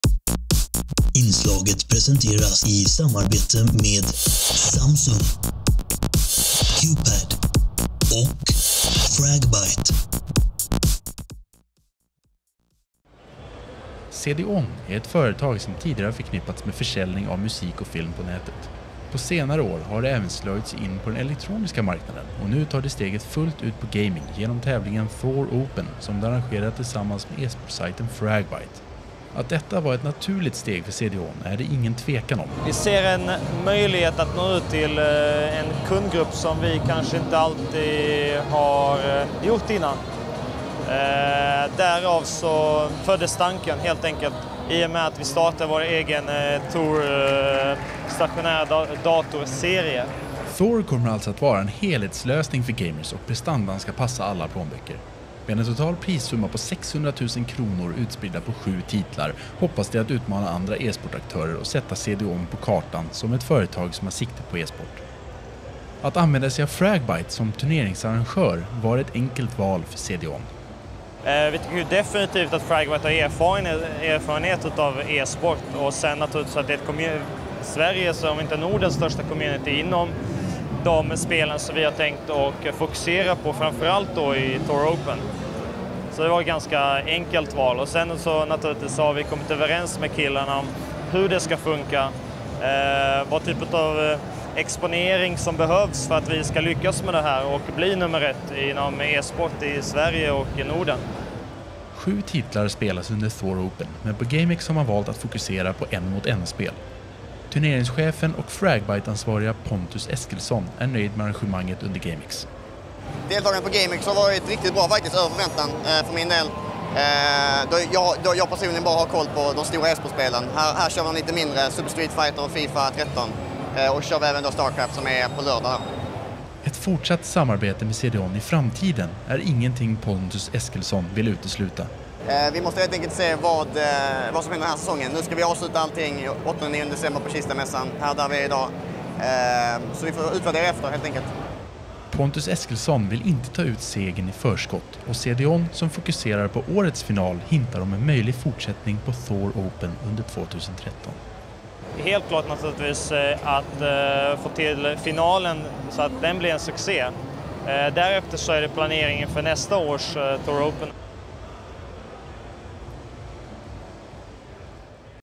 Reportage med CDON om Thor Open på GameX 2012
Reportage med Edin Turcinhodcic om Thor Open.